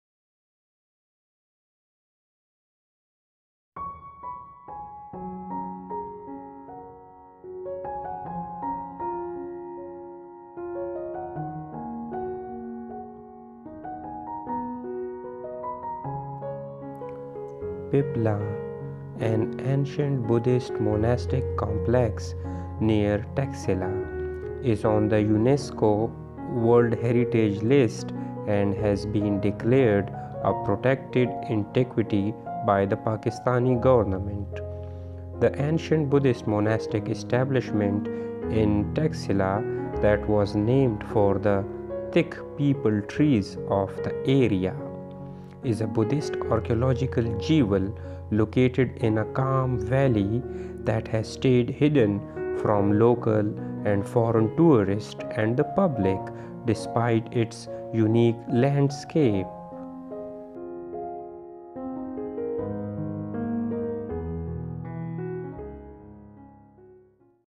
Pipplan- UNESCO world heritage Buddhist site, Gandhara
Piplan has been added to the Unesco World Heritage List of the convention concerning the protection of the world’s cultural and natural heritage, while the Pakistani government has declared the site a ‘protected antiquity’ under the 1975 antiquities act.
According to Mr Lone, a now dilapidated stupa in a cell was discovered in the southeast corner of the monastery. The floor of the stupa and the cell in which it stands is around 2ft below the rest of the monastery, leaving little doubt among the archaeologists and researchers that the stupa was originally built in one of the cells of the earlier, Kushan-era monastery and then incorporated into the later one.
A still intact 8ft high stupa stands in a chamber of the monastery. It was tradition to build a memorial stupa inside a cell after the prinirvana (death of a venerable monk). This stupa rises to three diminishing tires, the topmost surmounted by a dome and originally crowned by an umbrella, decorated with lotus rosettes and images of Buddha, while at the base of the dome is a series of seated Buddhas. However, due to a lack of preservation and conservation, the stupa is in shambles.